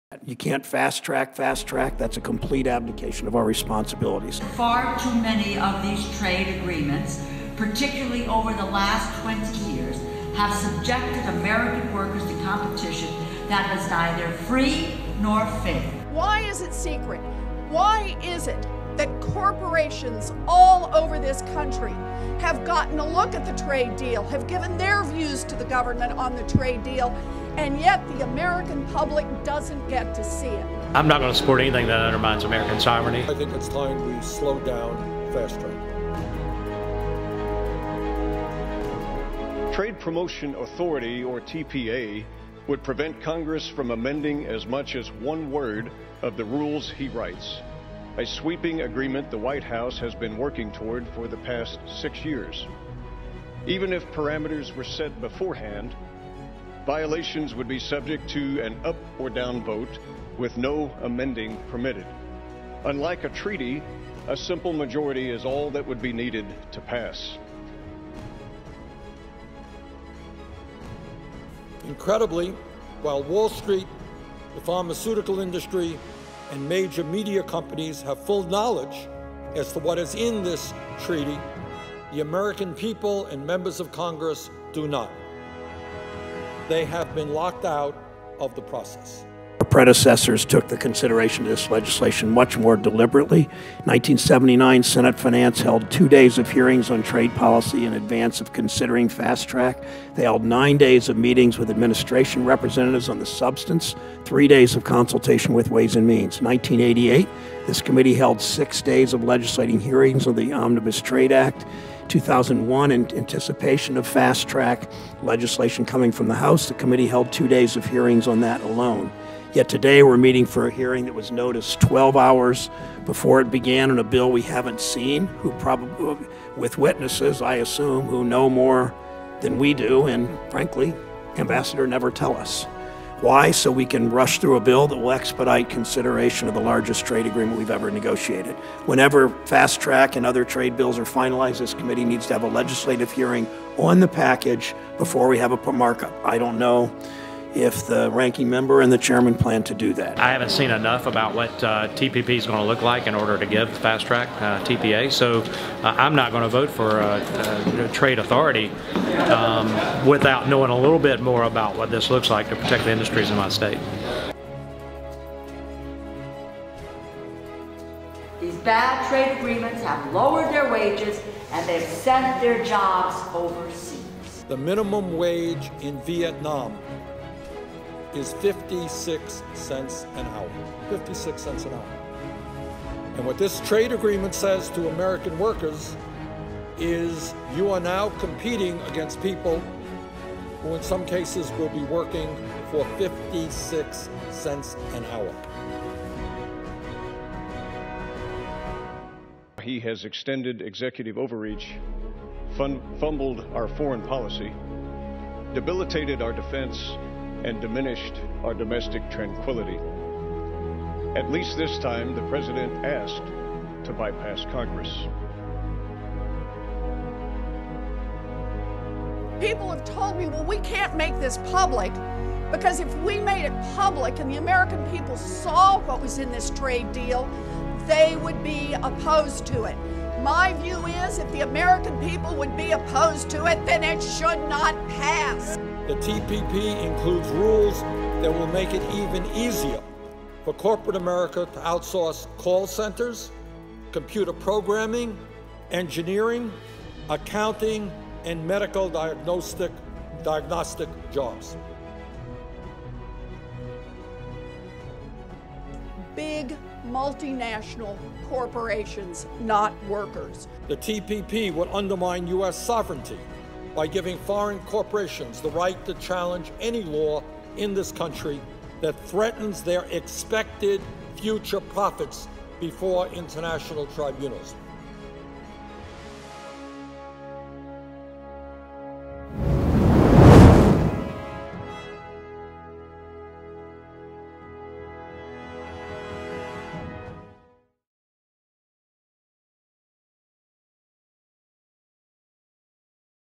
Questions Regarding Fast Track Answered by Congress
Democrats, Independents, and Republicans are all on the same page on this issue: don't give President Obama Fast Track. Of course their reasons are all different, but the matter of the fact is that Fast Track will destroy our economy. If you don't know what Fast Track is or how it will affect your personally, get answers directly from members of Congress.